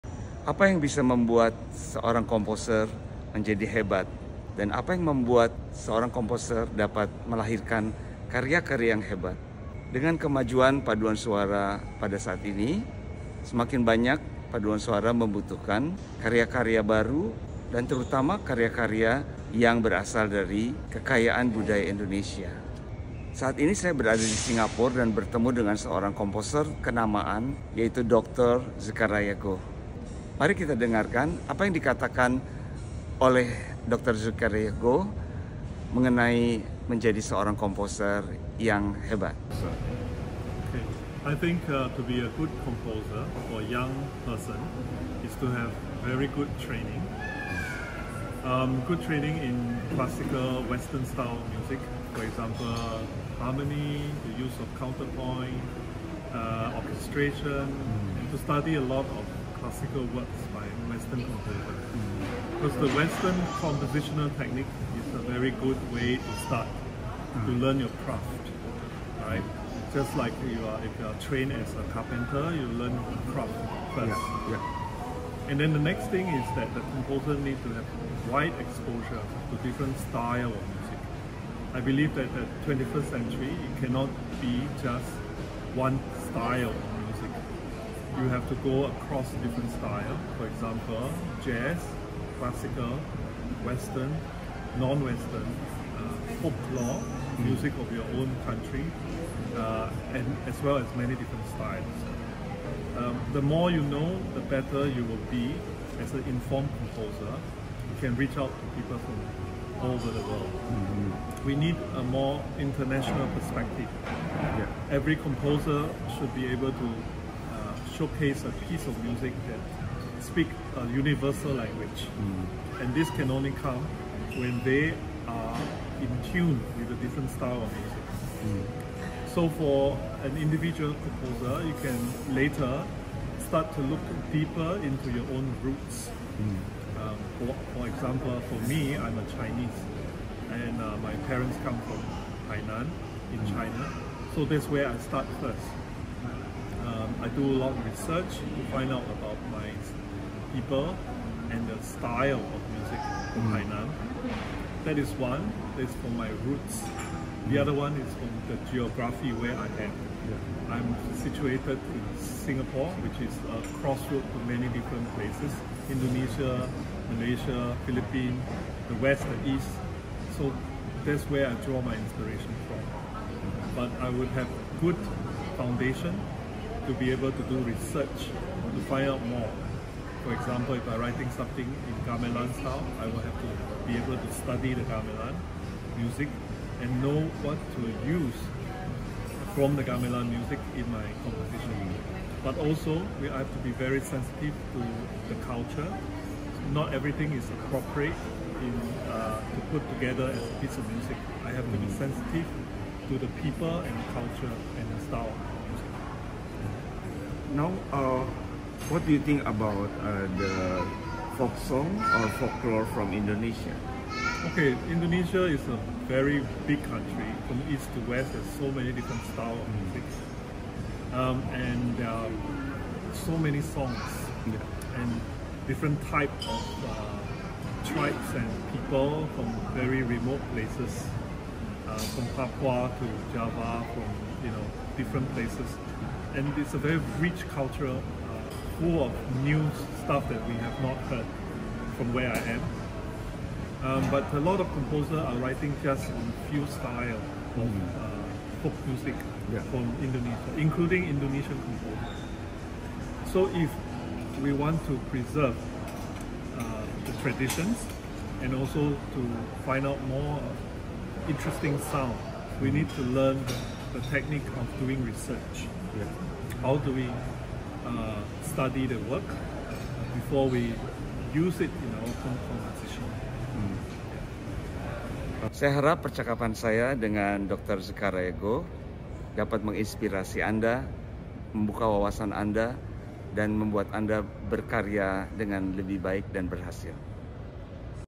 [VIDEO PENDEK TK #5] Inspiration for Composer! ft. Dr. Zechariah Goh
Inspiration for Composer!
Thank you dear friend Dr. Zechariah Goh, Professor at Nanyang Academy of Fine Arts, Singapore 😍